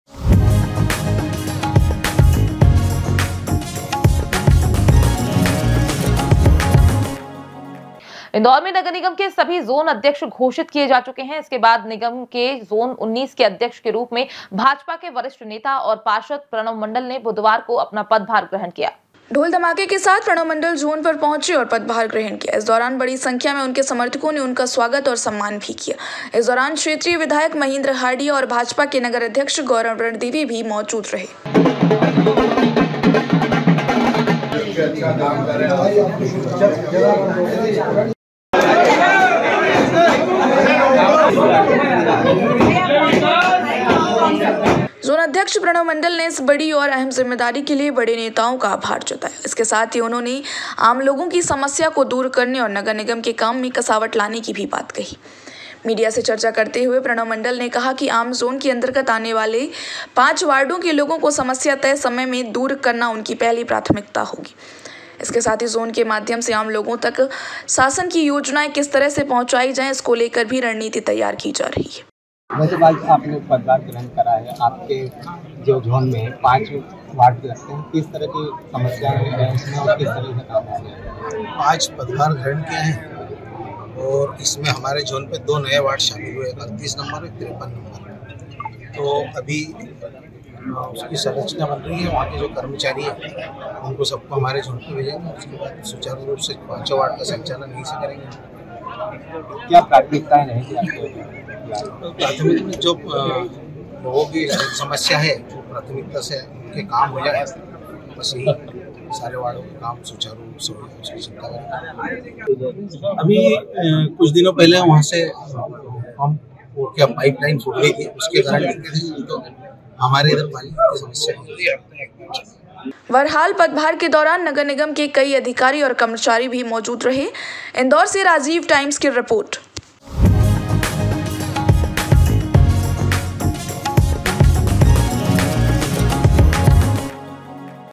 INDORE वार्ड 41 के पार्षद प्रणव मंडल ने ग्रहण किया पदभार#indorenews
INDORE वार्ड 41 के पार्षद प्रणव मंडल ने ग्रहण किया पदभार